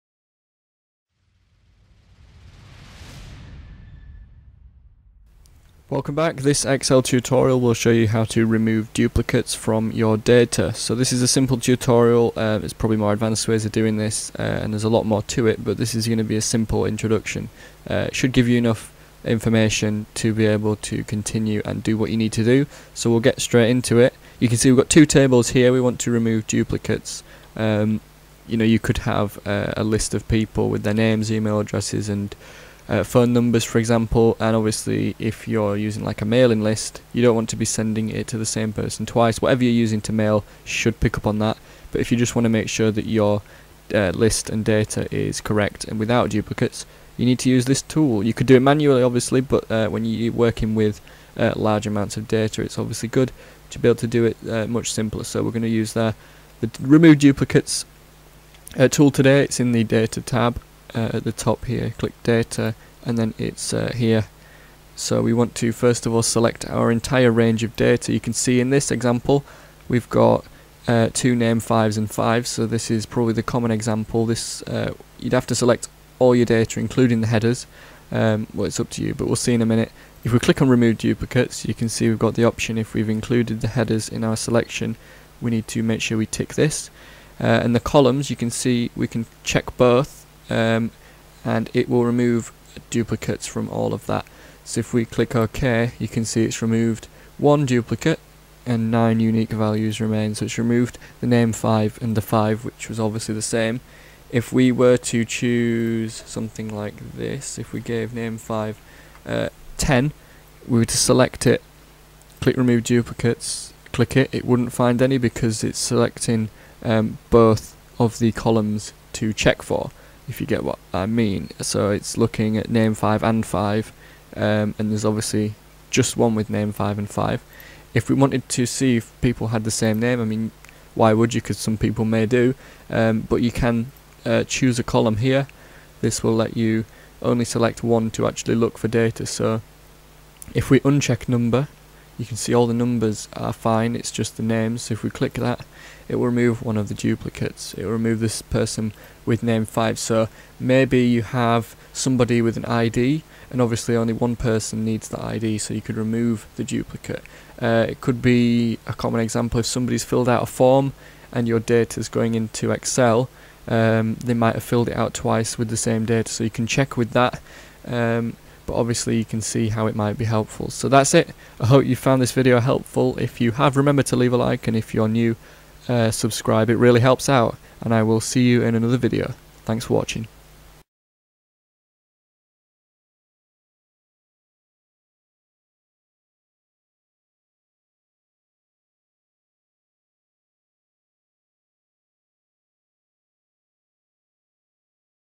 How to Remove Duplicates From Data in Microsoft Excel
This video will show you how to remove duplicates from your data in Microsoft Excel. Duplicates can be in any of the columns selected and this will work great if you are working with a database of profiles for example, maybe with a of email addresses in a mailing list. It's quite a simple tutorial, but there are areas of the remove duplicates command that can confuse some, hopefully I'll be able to help you today!